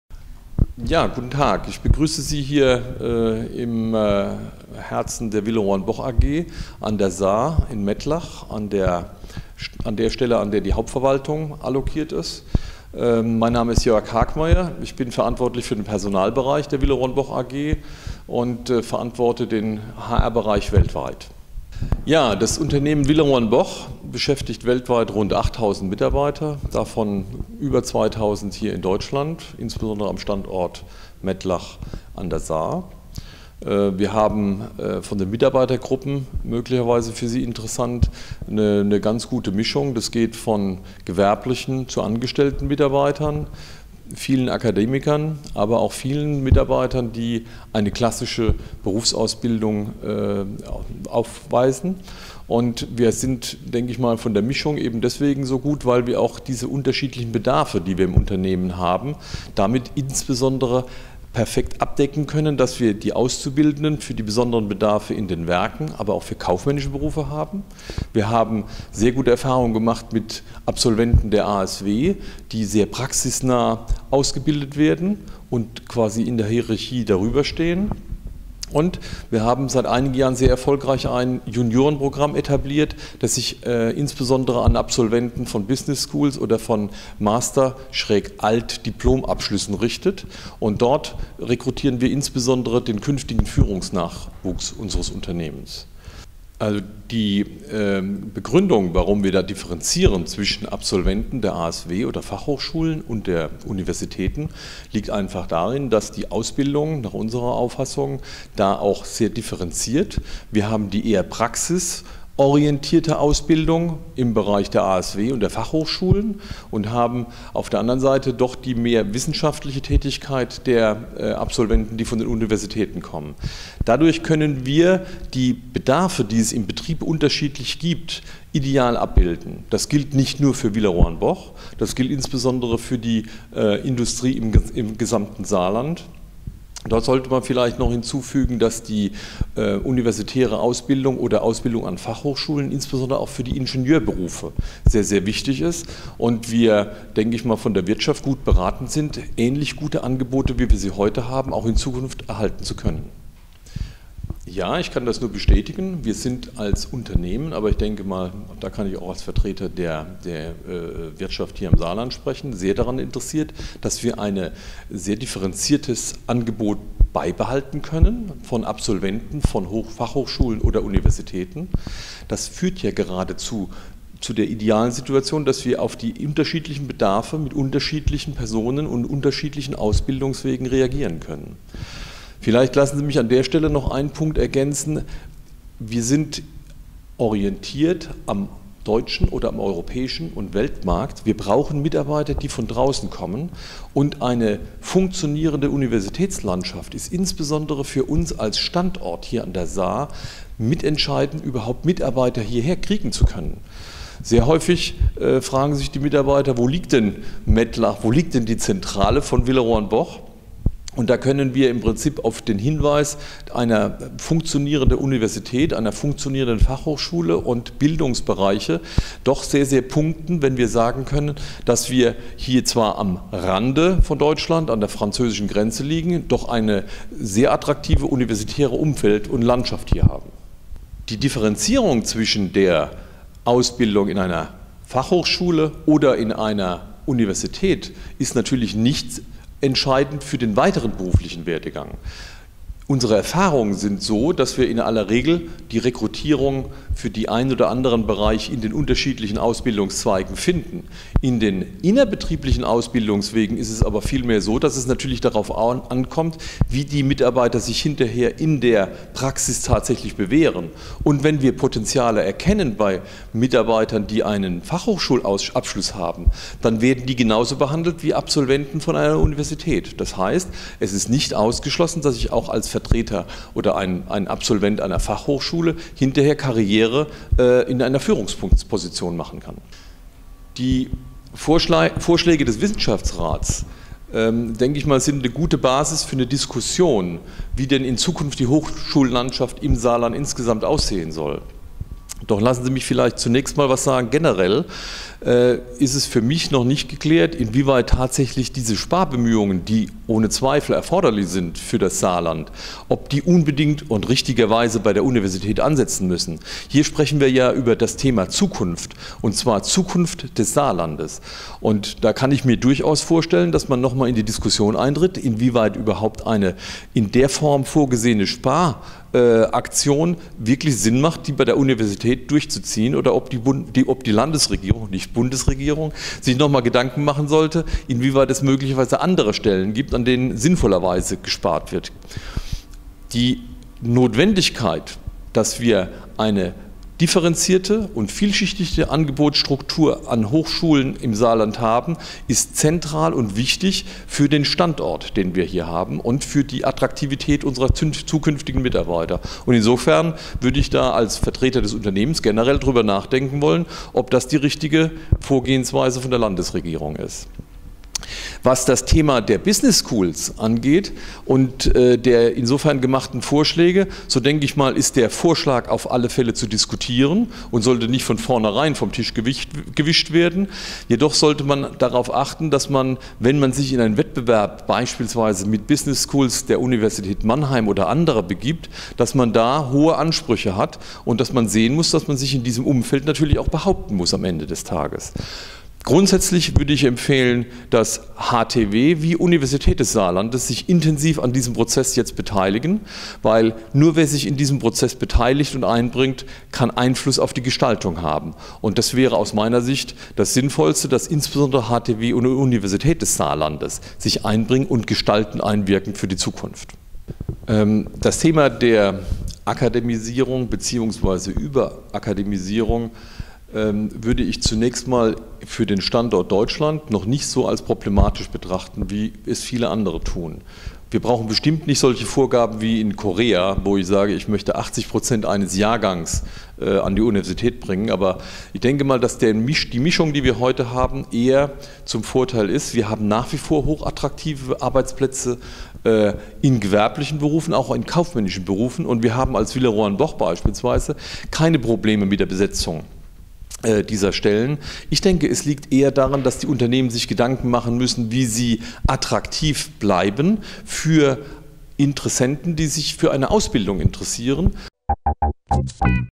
Statement von Herrn Hagmaier zum Beschluss des Wissenschaftsrates
Herr Hagmaier, Personalleiter der Velleroy & Boch AG, äußert sich zu den aktuellen Beschlüssen des Wissenschaftsrates im Rahmen der Sparpläne der saarländischen Landesregierung